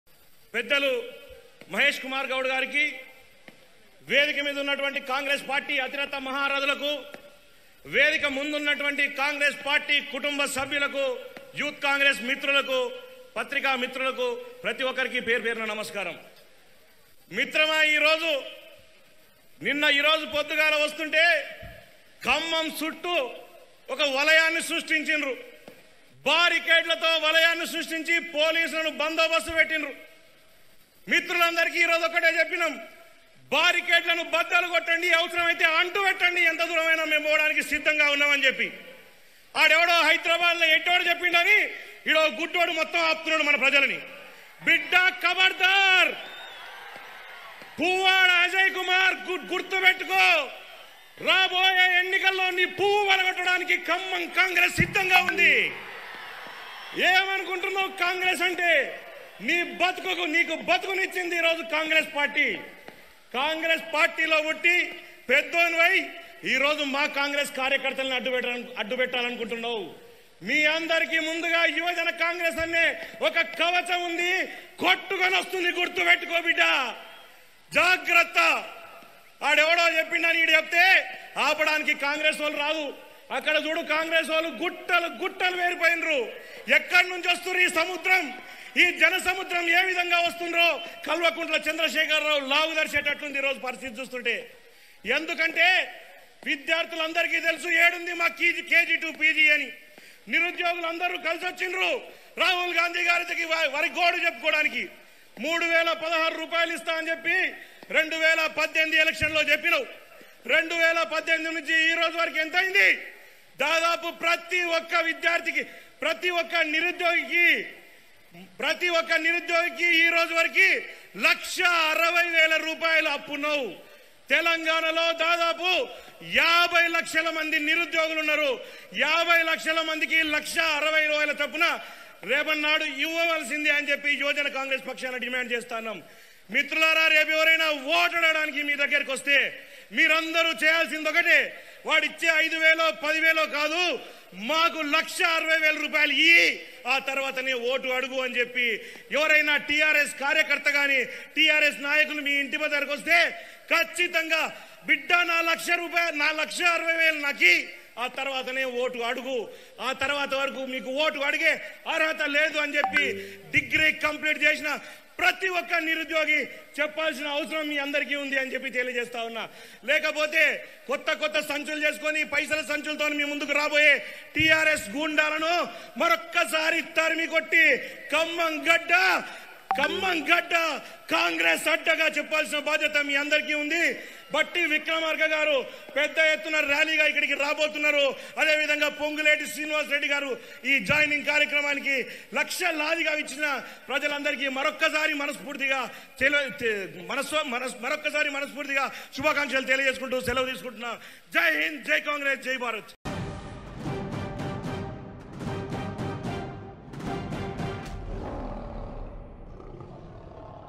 పువ్వాడ అజయ్ కుమార్ కి మాస్ వార్నింగ్| Tcongress Leader Shiva Sena Reddy Fires On Puvvada Ajay Kumar
Leo Politics

Leo Health

Leo Entertainment

Leo Devotional